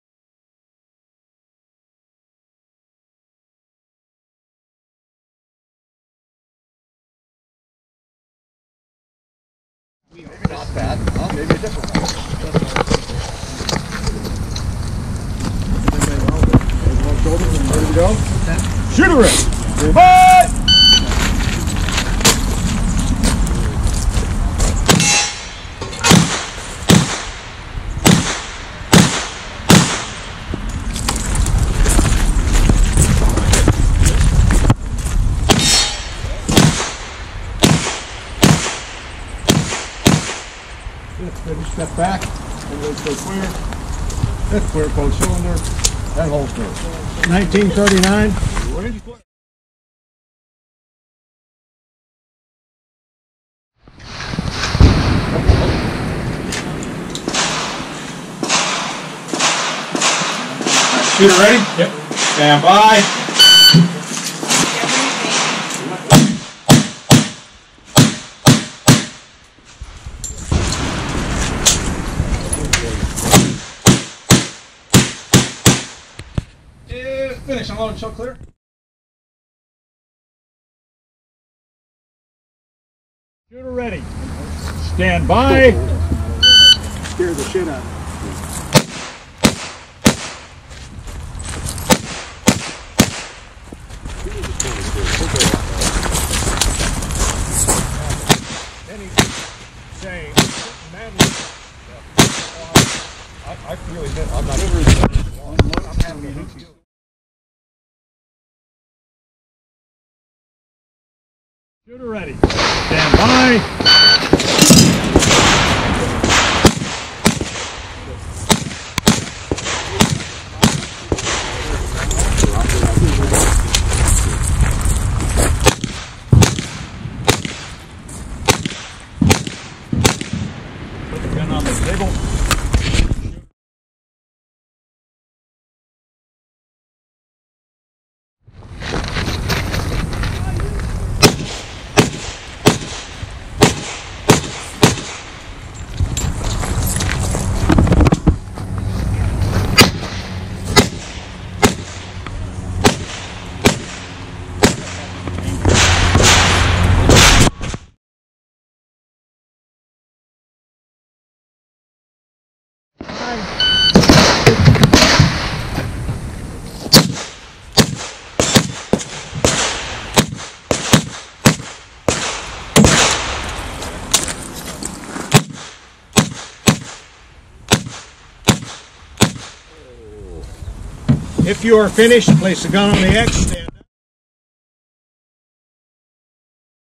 ASI 5 Feb 2022 Paul Bunyan with my S&W 325 Thunder Ranch 45ACP Revolver!
Holy Cow, Millard Hixson did a great job on the trigger of my revolver!  It's like shooting a new gun and I'm gonna have to get used to how much faster I can potentially shoot it now!